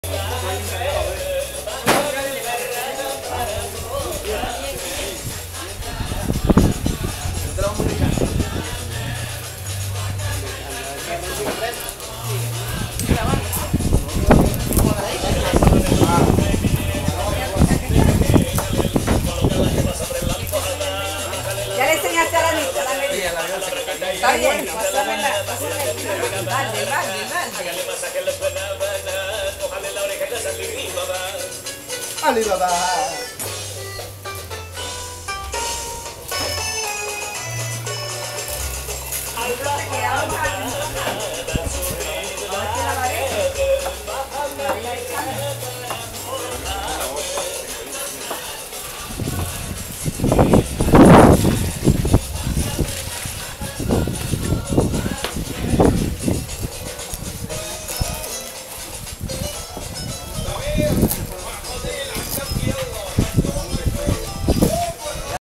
Diriá 28 Jun 2016 Amigas de Gpo San Pedro preparan comida
Mujeres que se dedican a la elaboracion  de comida para las festividades de la celebracion de San Pedro Apostol.

Por años estas personas han venido apoyando al grupo san pedro apostol para esta actividad.